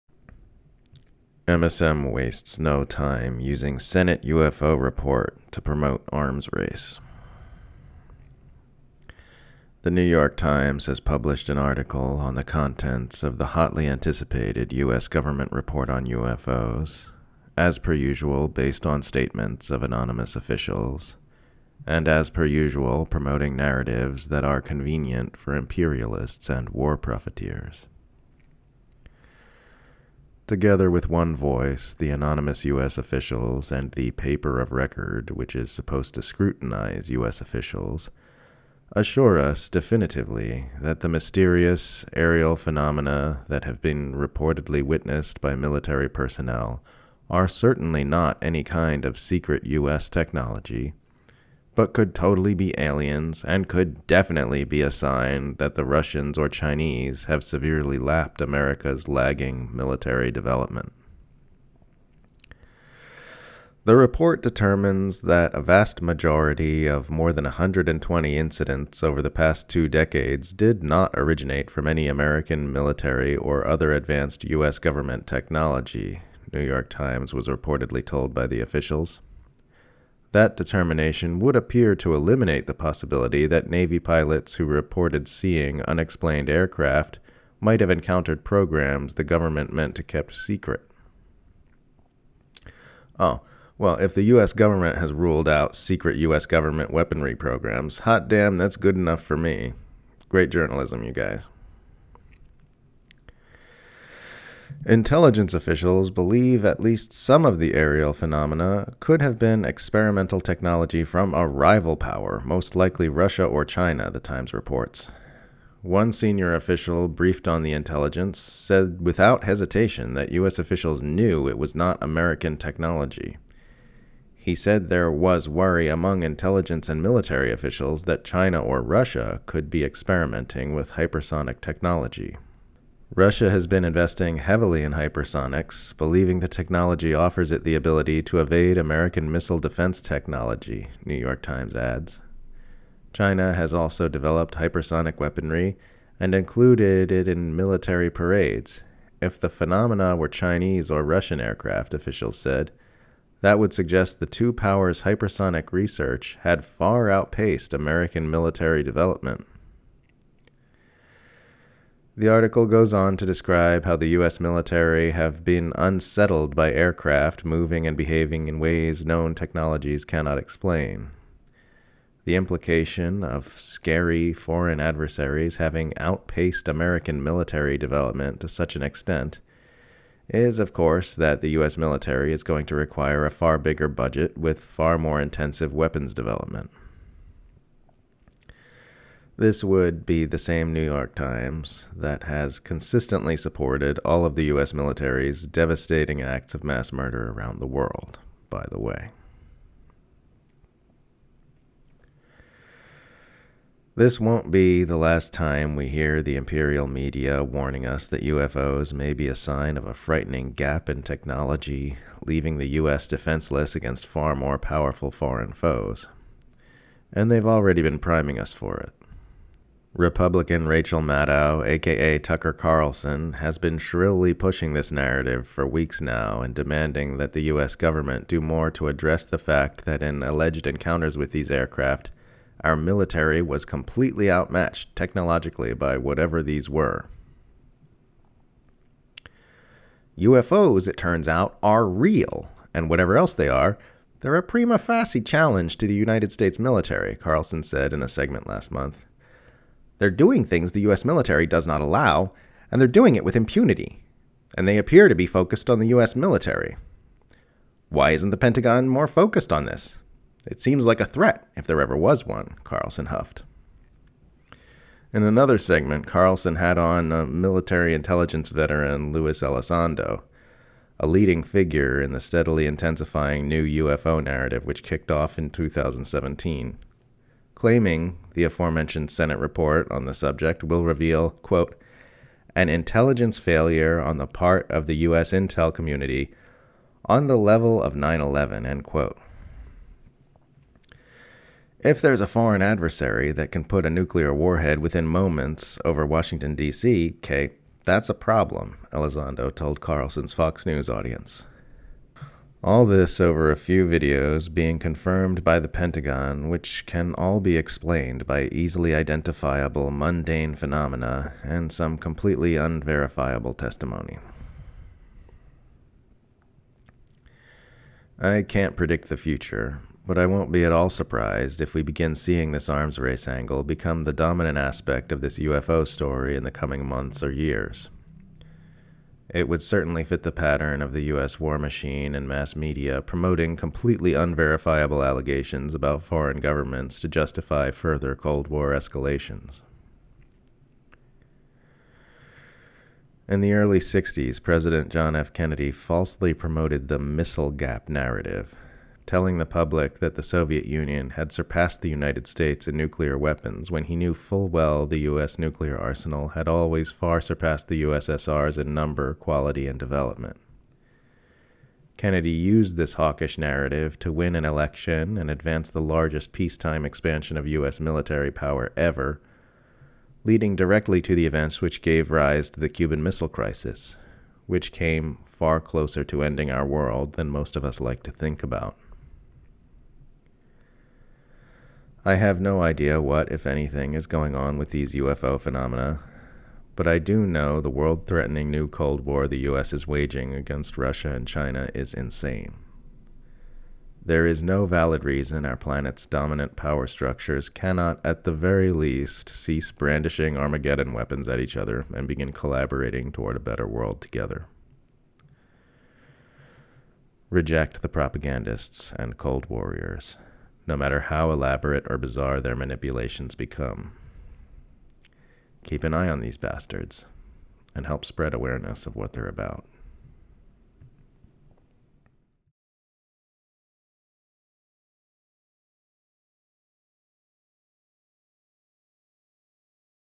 MSM Wastes No Time Using Senate UFO Report To Promote Arms Race
The New York Times has published an article on the contents of the hotly anticipated US government report on UFOs, as per usual based on statements of anonymous officials, and as per usual promoting narratives that are convenient for imperialists and war profiteers.

Together with one voice, the anonymous US officials and the "paper of record" which is supposed to scrutinize US officials assure us definitively that the mysterious aerial phenomena that have reportedly been witnessed by military personnel are certainly not any kind of secret US technology, but could totally be aliens and could definitely be a sign that the Russians or Chinese have severely lapped America's lagging military development.

Reading by Tim Foley.